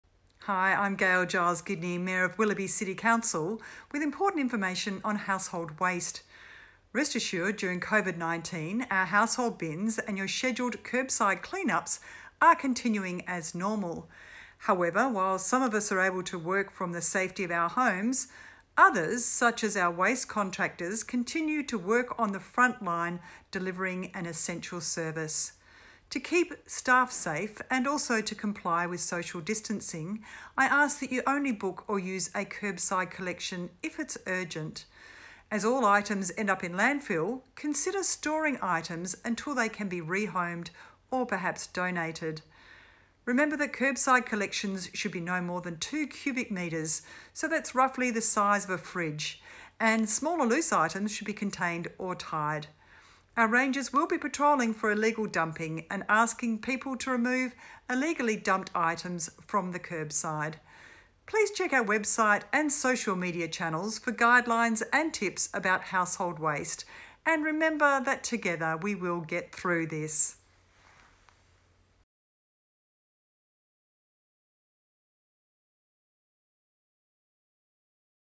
Waste Management
Mayor Gail Giles-Gidney explains how we can all work together to remove waste efficiently and help keep our staff safe during COVID-19.